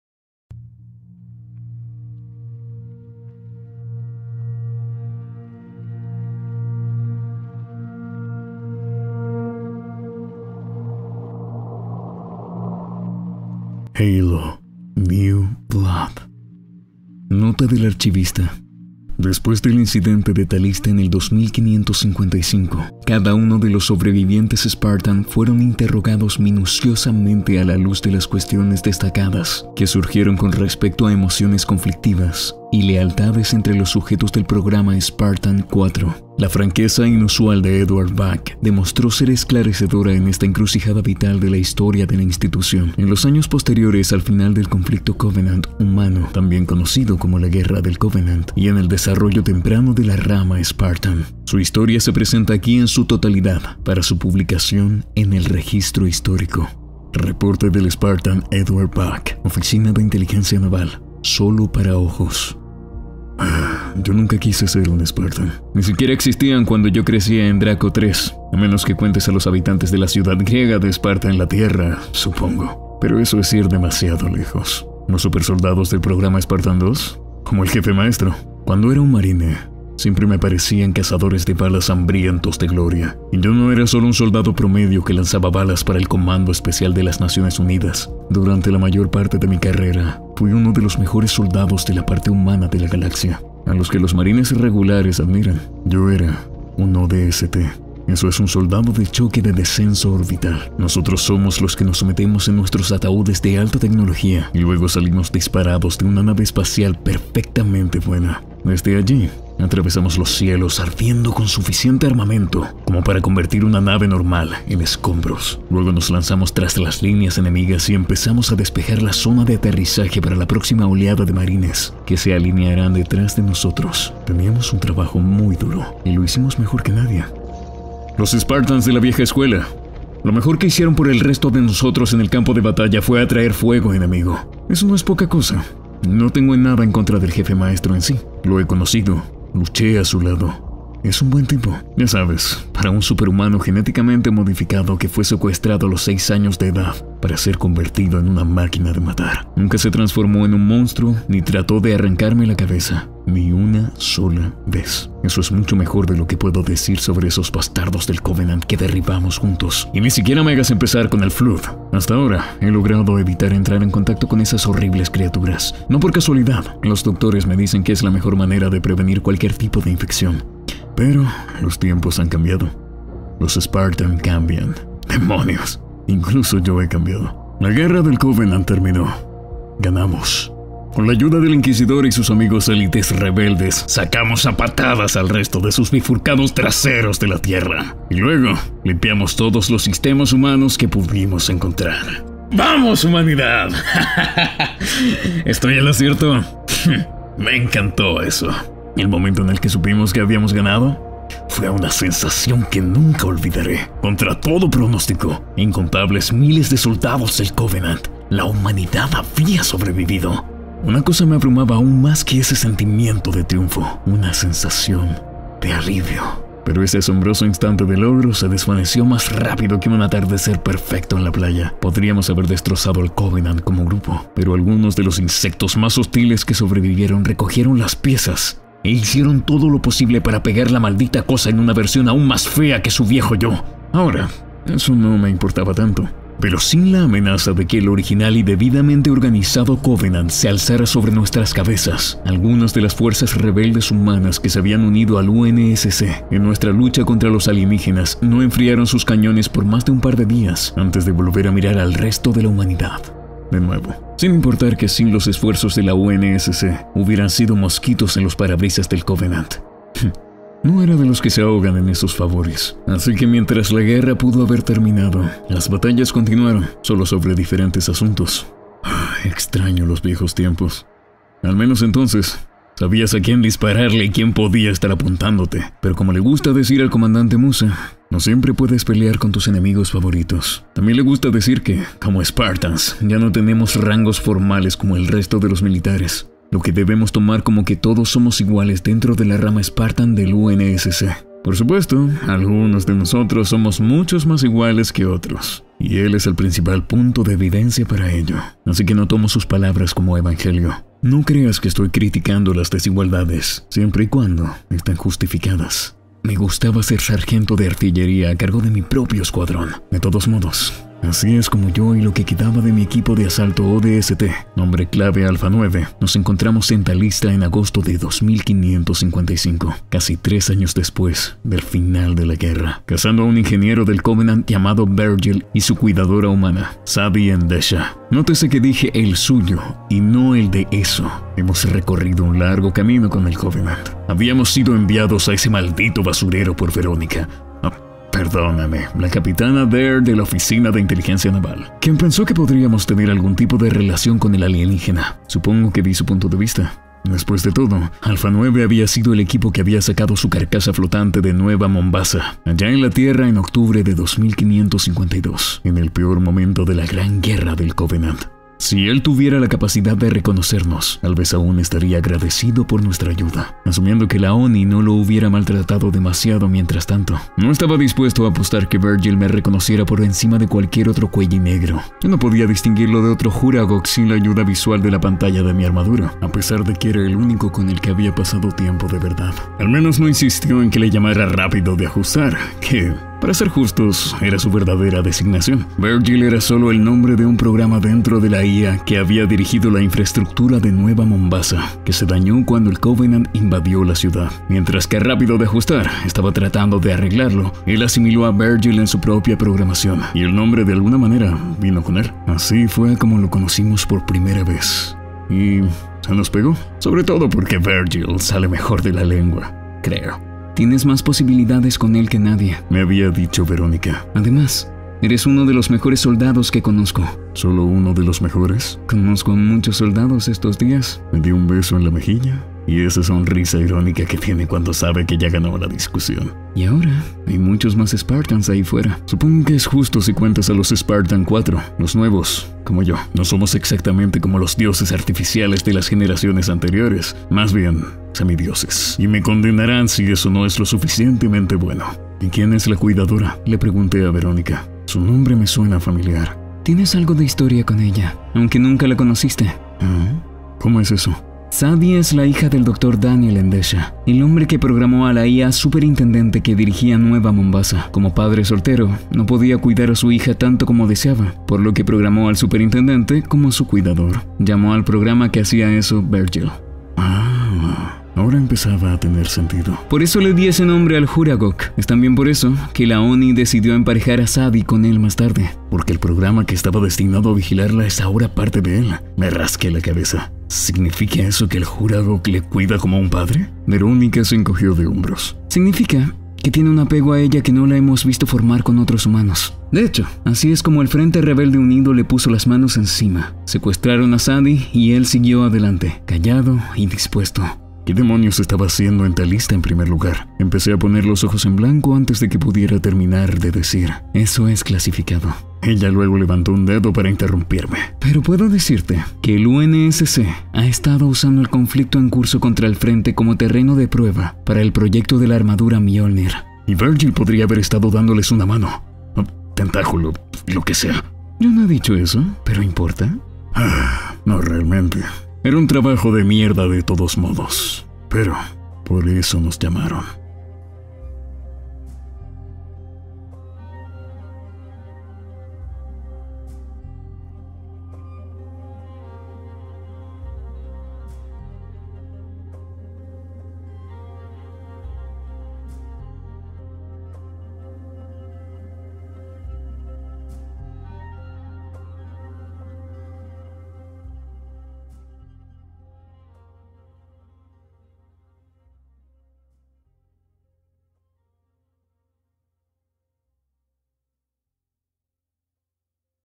Halo "New Blood" - Introducción (Audio Relato)
Introducción a la novela "Halo - New Blood" de Matt Forbeck, en esta ocasión en formato de audio relato. Si gustas de este contenido comenta que otros relatos te gustaría ver relatados.